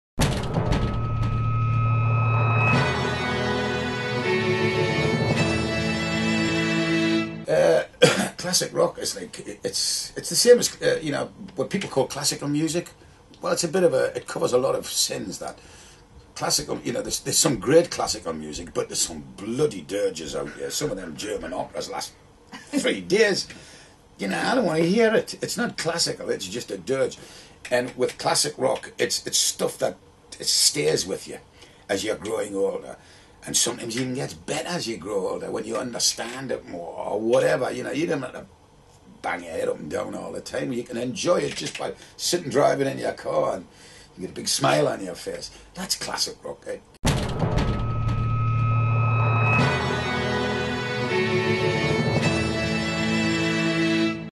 Brian Johnson AC/DC Interview Teaser
Hear Brian Johnson from AC/DC in conversation with Leona Graham this Sunday 1st August 2010 at 6pm (GMT) on Absolute Classic Rock.

He talks about his new book 'Rockers and Rollers', the recent AC/DC tour and his view on the state of the music industry today.